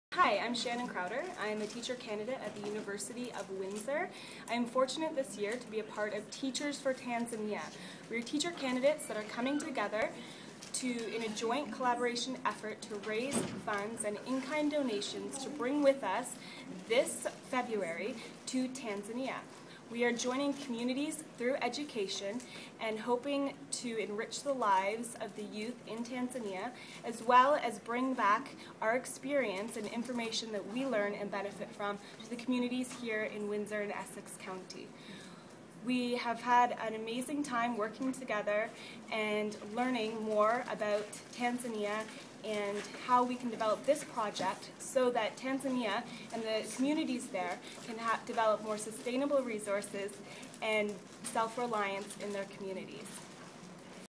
Windsor Students Gear Up For Tanzania Trip
Shannan Crowder speaks about an upcoming trip by Teachers For Tanzania